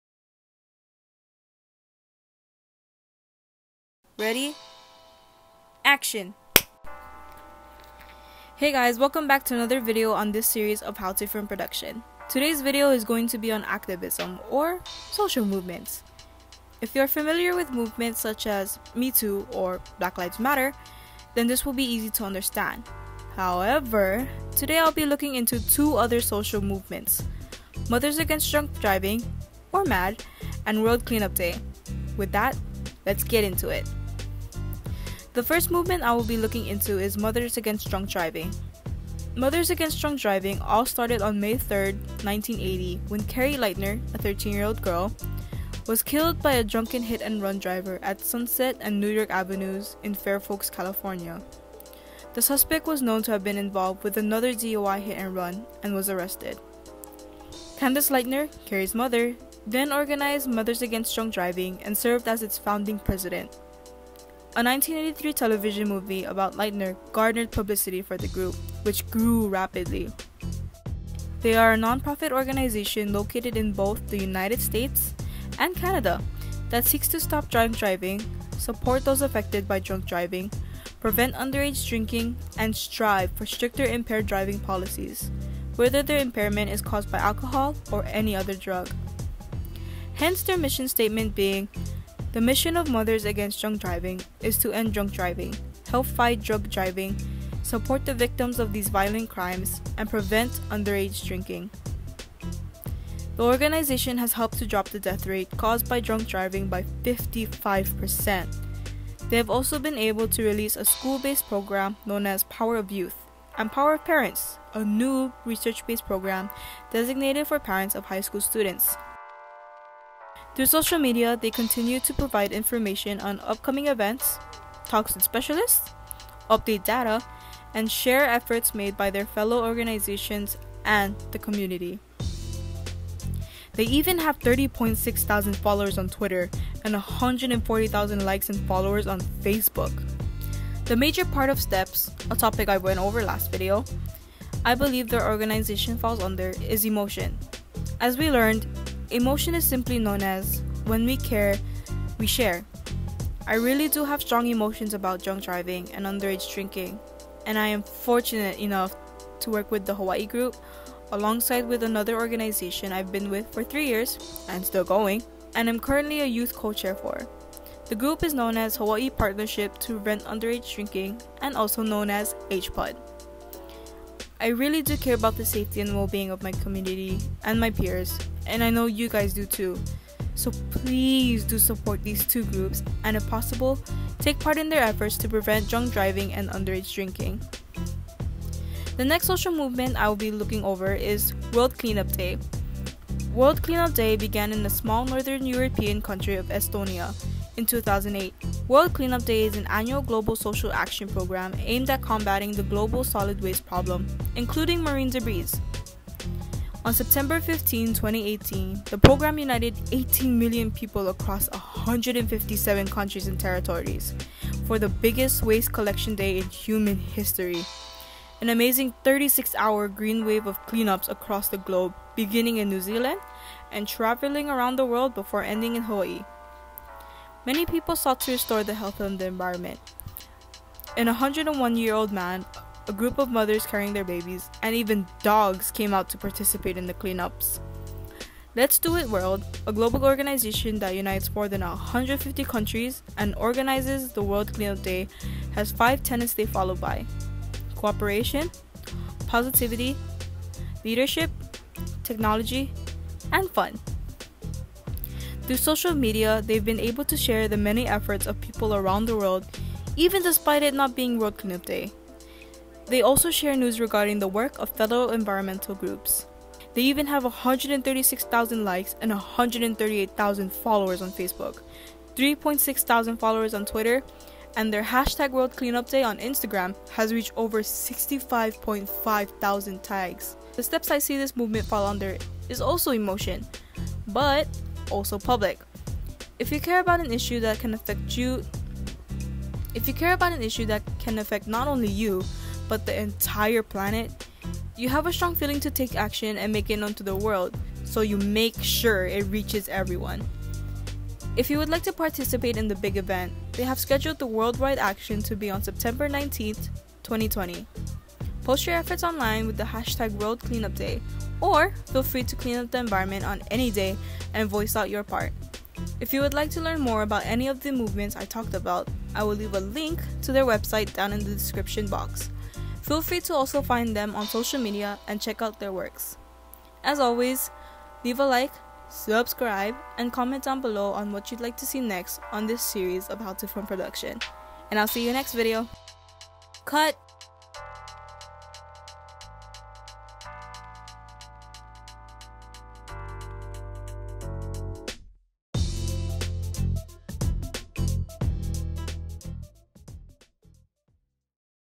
Activism
Hey guy! Today’s video is going to be about activism and social media. The two movements I will look over is Mothers Against Drunk Driving (MADD) and World Clean Up Day.

Feel free to also let me know what other social movements you guys know!